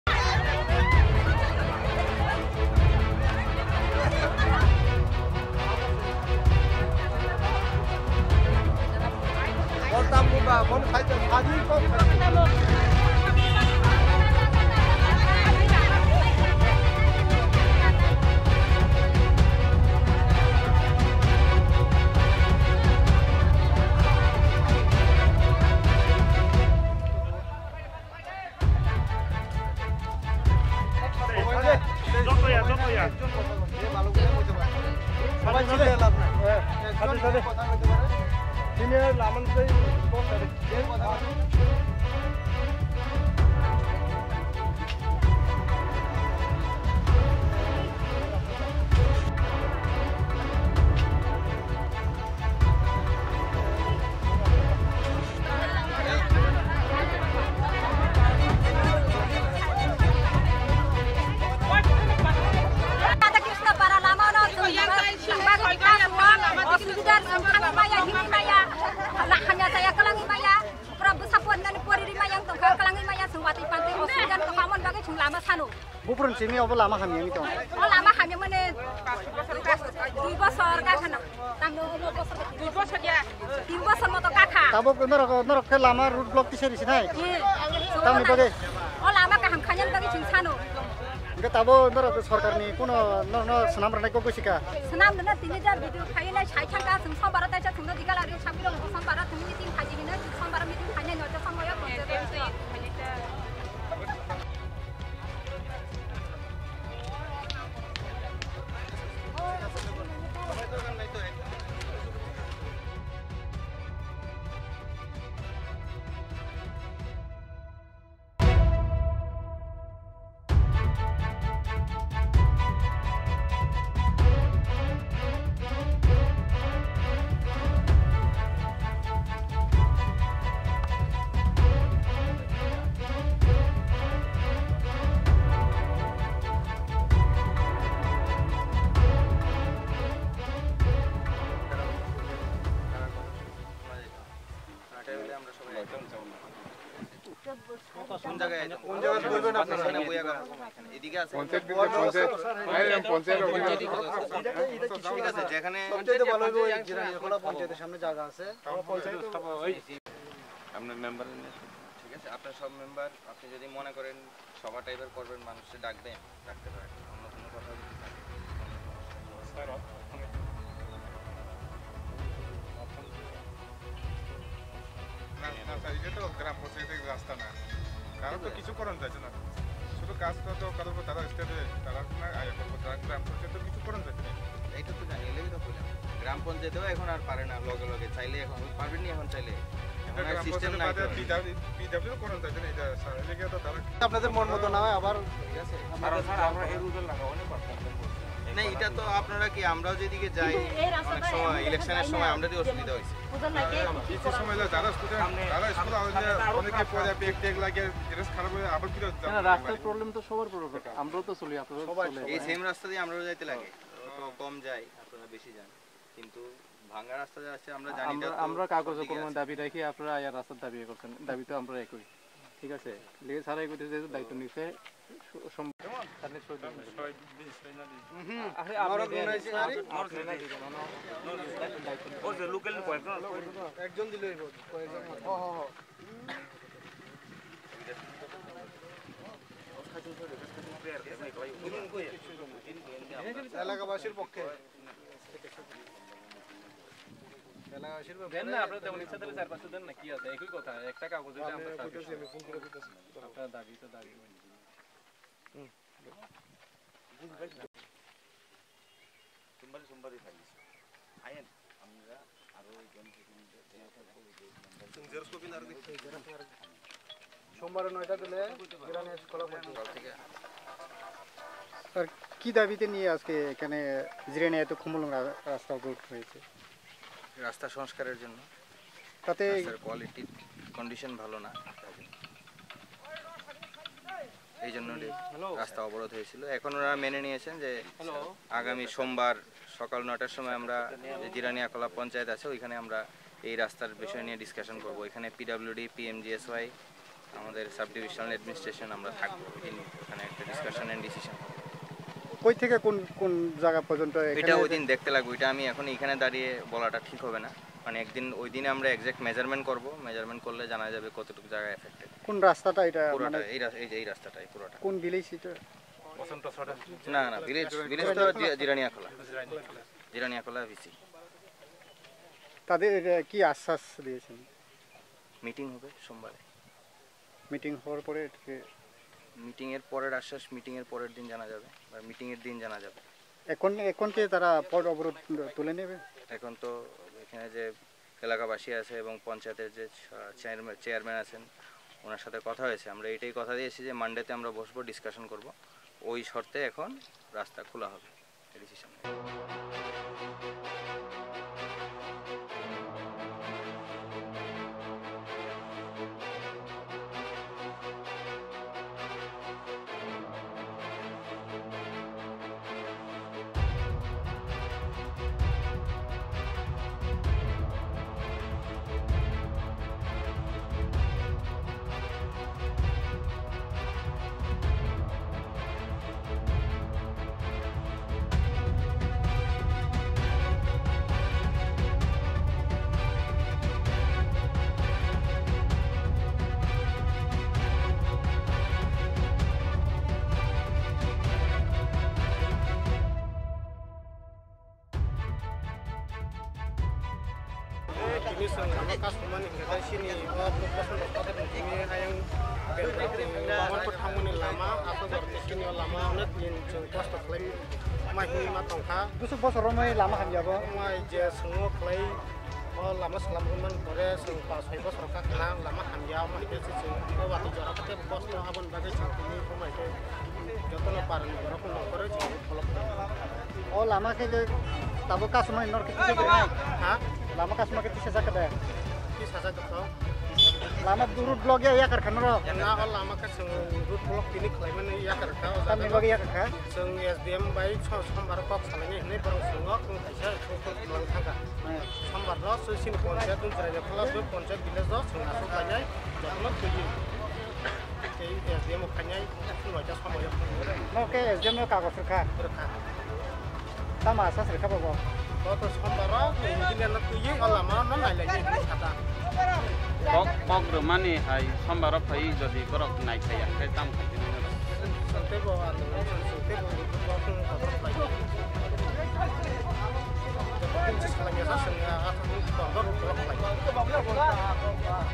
Jirania to Khumulwng Road Block Six nos Villager's II Demand Road Maintanance II
Jirania to Khumulwng Road Block Six nos Villager's # Demand Road Maintanance Basanta Sadar Para To Radha Das Para